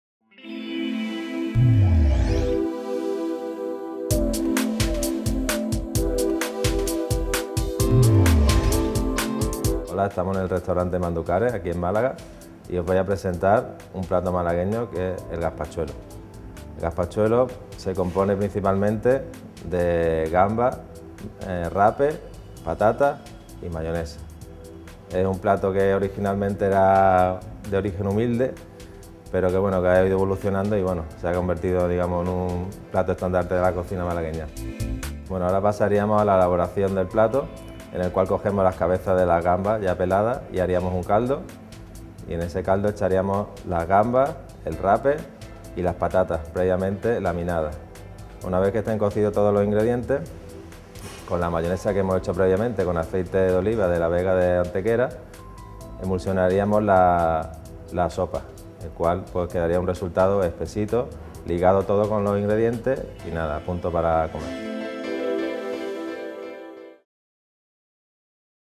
Gazpachuelo
INGREDIENTES:
Gambas, rape, patatas, y mayonesa

ELABORACIÓN:
Ponemos agua en una olla y con las cabezas de las gambas ya peladas, hacemos un caldo al que le añadimos un chorrito de aceite, también le añadimos las gambas, el rape y las patatas previamente laminadas. Una vez cocidos todos los ingredientes con la mayonesa que hemos hecho con aceite de oliva, la mezclamos con el caldo poco a poco para que no se corte y ya tenemos la sopa lista. Se le puede añadir un chorrito de limón una vez servida, al gusto. Y a disfrutar.

Restaurante Manducare:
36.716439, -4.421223